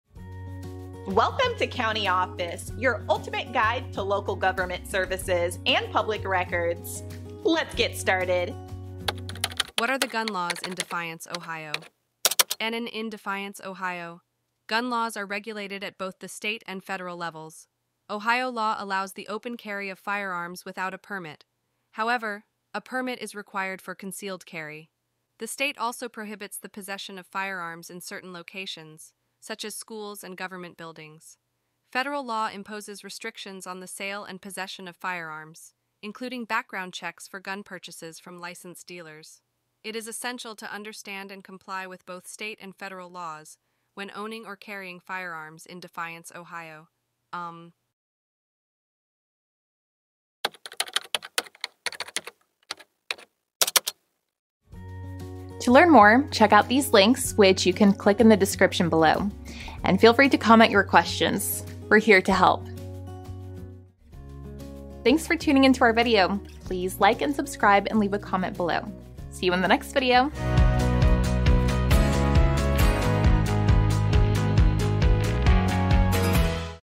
What Are the Gun Laws In Defiance, Ohio? - CountyOffice.org
What Are the Gun Laws In Defiance, Ohio? In Defiance, Ohio, gun laws are regulated at both the state and federal levels. Ohio law allows the open carry of firearms without a permit but requires a permit for concealed carry. Certain locations, such as schools and government buildings, prohibit the possession of firearms. Federal law imposes restrictions on the sale and possession of firearms, including background checks for gun purchases from licensed dealers. Understanding and complying with both state and federal laws is crucial when owning or carrying firearms in Defiance, Ohio.

Join us for an in-depth look at the gun laws in Defiance, Ohio, in this informative video. Learn about the regulations surrounding open and concealed carry, as well as the restrictions on firearm possession in specific locations. Discover the importance of adhering to both state and federal laws to ensure lawful ownership and carrying of firearms in Defiance, Ohio.

He